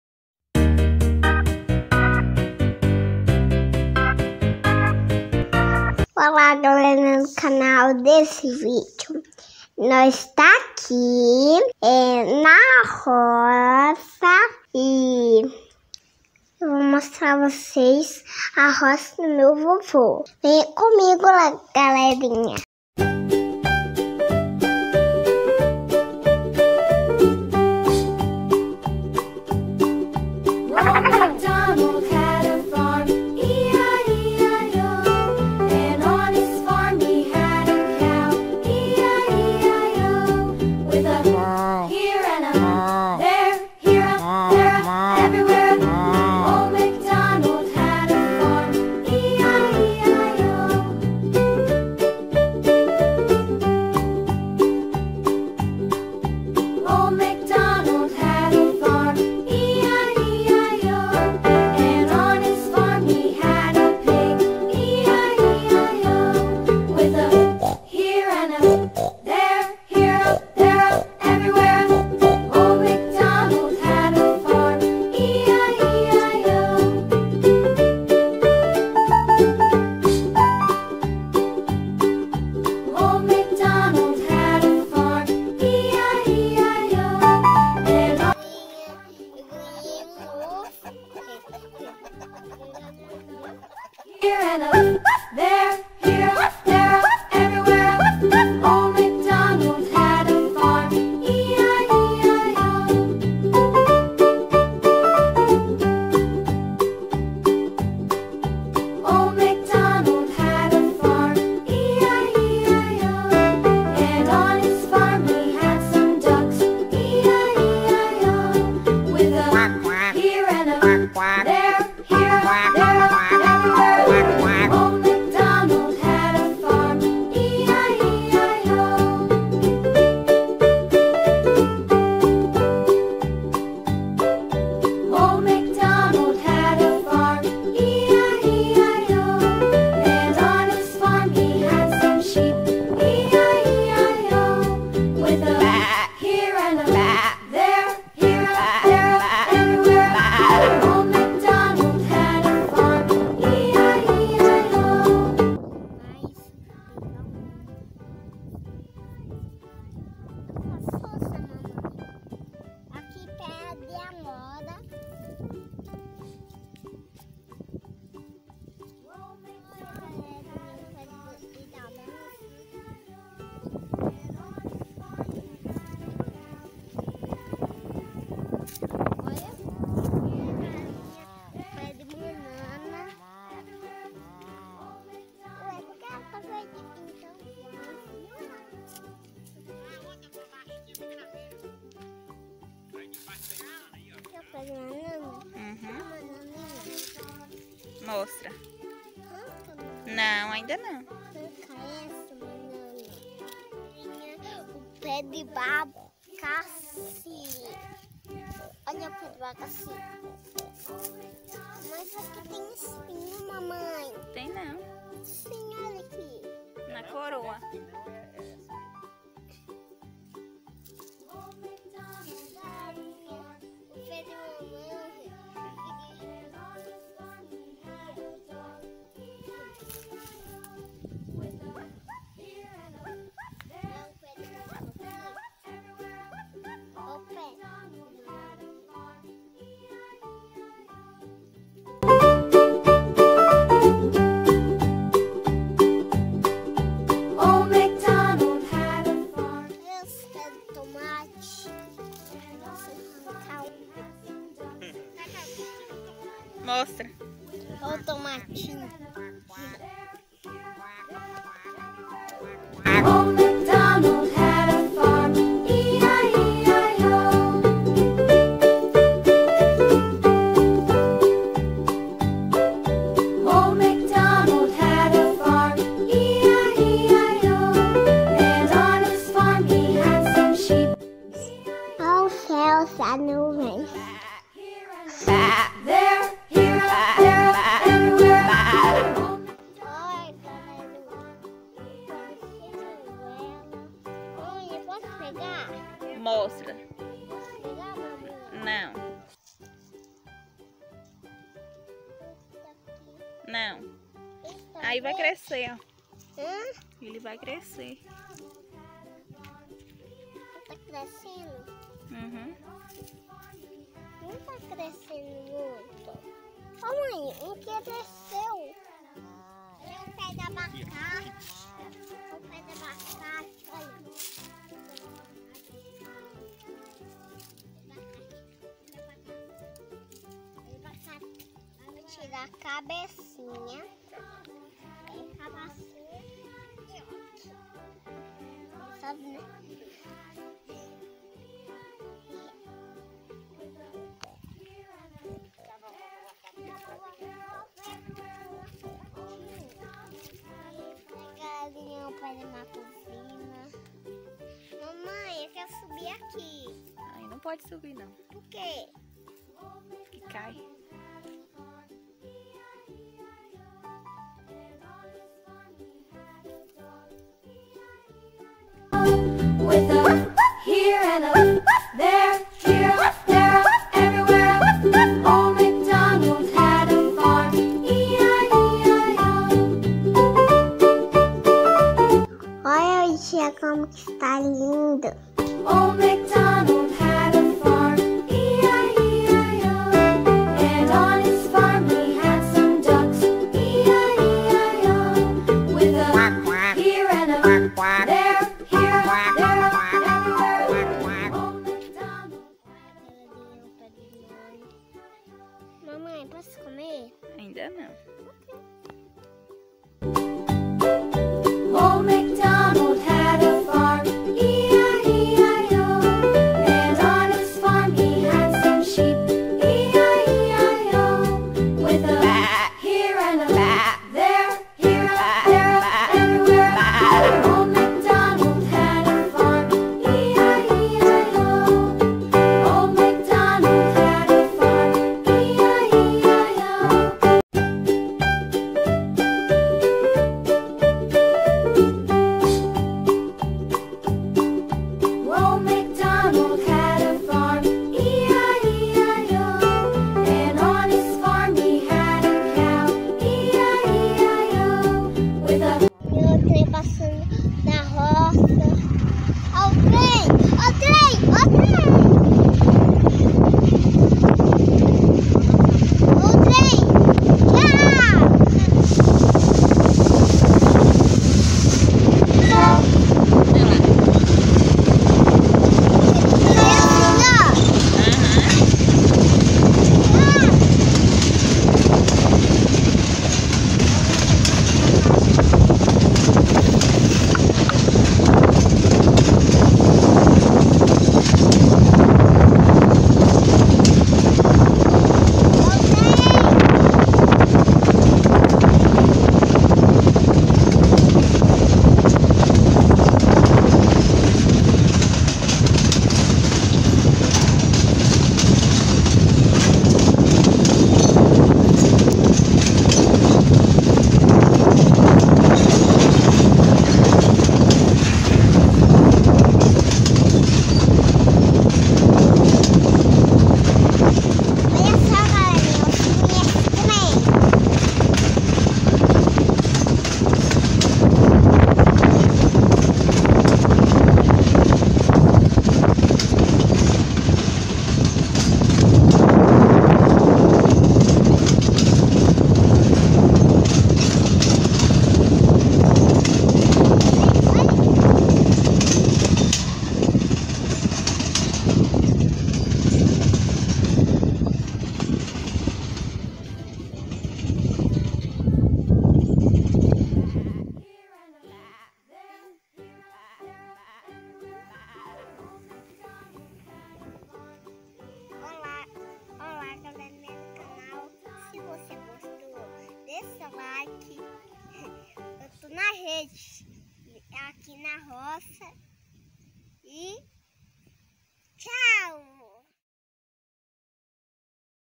Tour pela fazenda do vovô
Oiii amiguinhos, confiram nesse vídeo, alguns dos meus momentos na fazenda do meu vovô.
Espero que gostem, pois preparei com muito carinho para vocês!

Beijinhos no coração e tchau tchau 😘